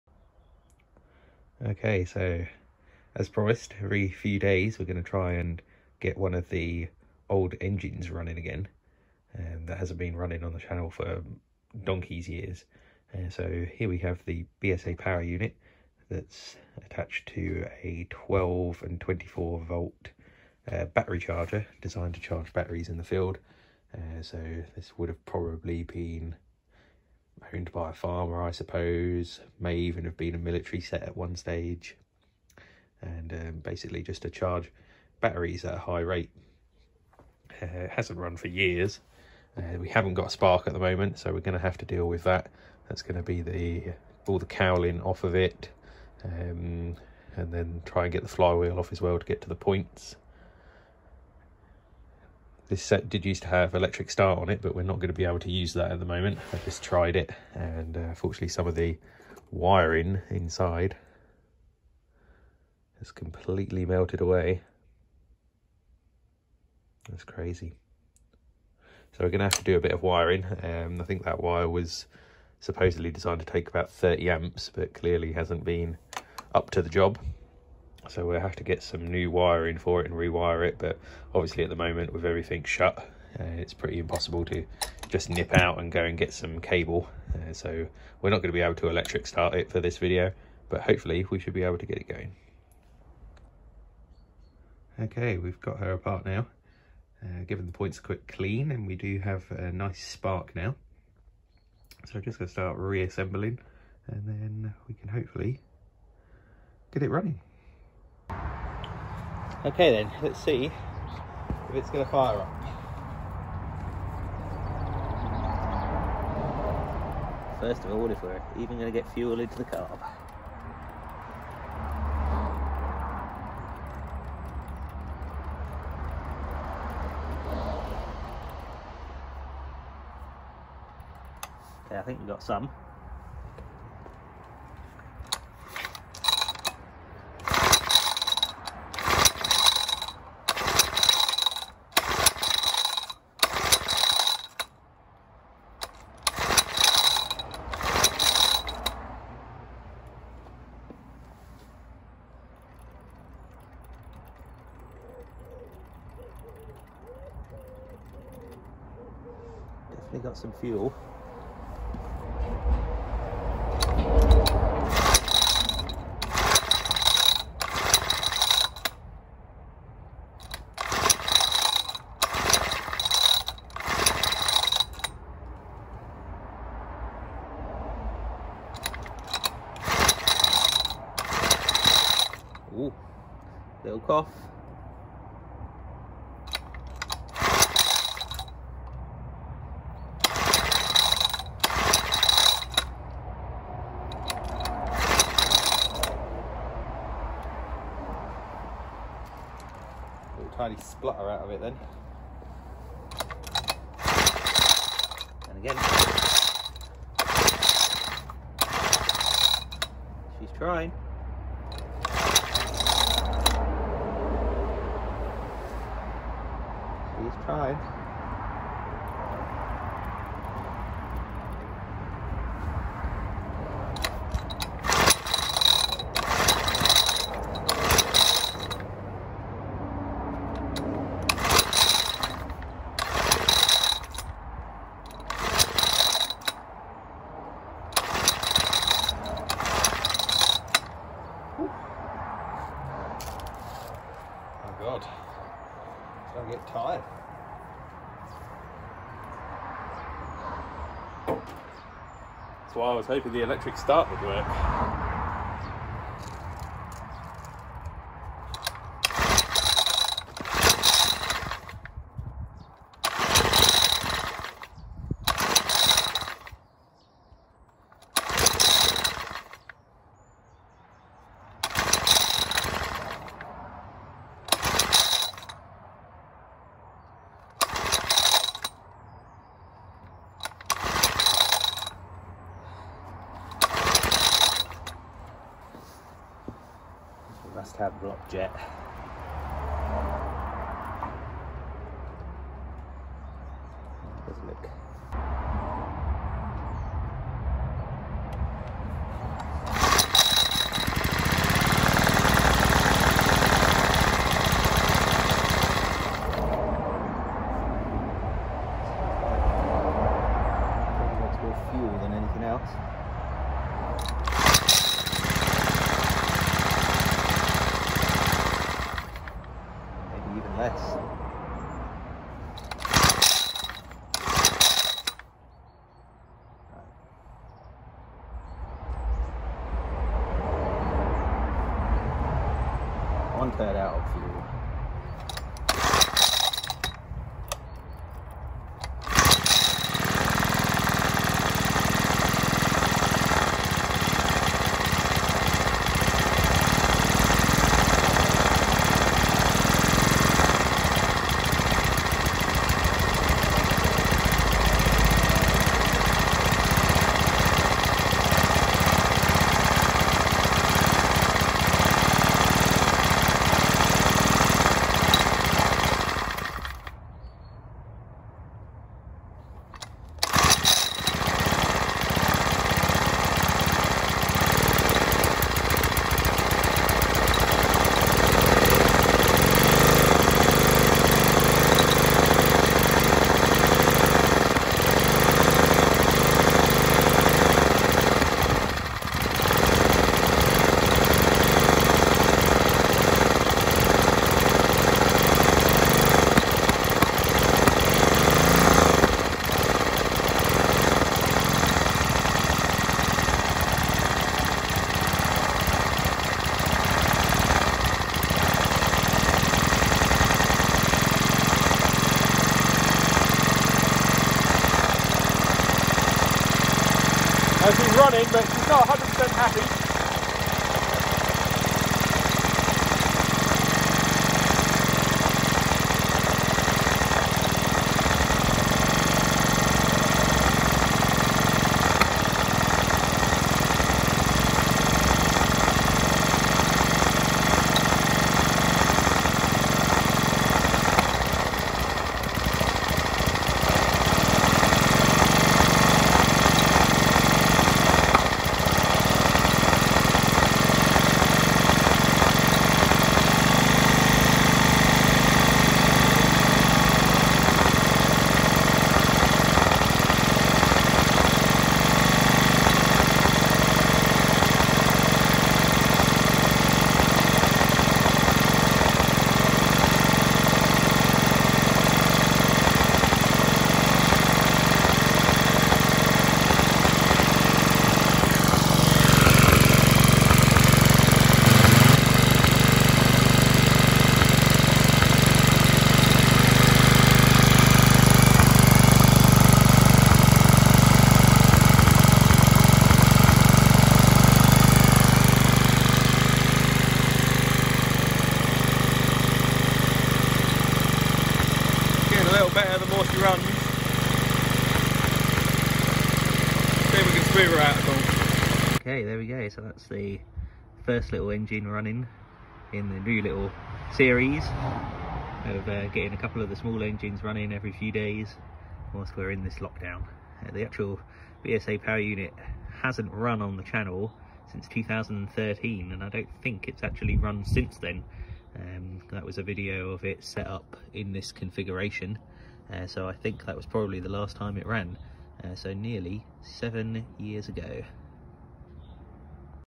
BSA Power Unit - Repair and first run in 7 years!
As promised during this unprecedented lock down in the uk we are going to use the time to get some of my long since forgotten stationary engines running!

Today we look at the BSA power unit. The engine is a 50cc BSA coupled to a 12-24 volt DC generator. These would have been used to rapidly charge flat batteries in the field. Unfortunately when it was last run it appears some wiring melted on the electric start side of the unit. With no access to suppliers this repair will have to wait.

The engine needs a good carb clean and also a new crankcase gasket as it had lost all its oil in the 7 year lay up!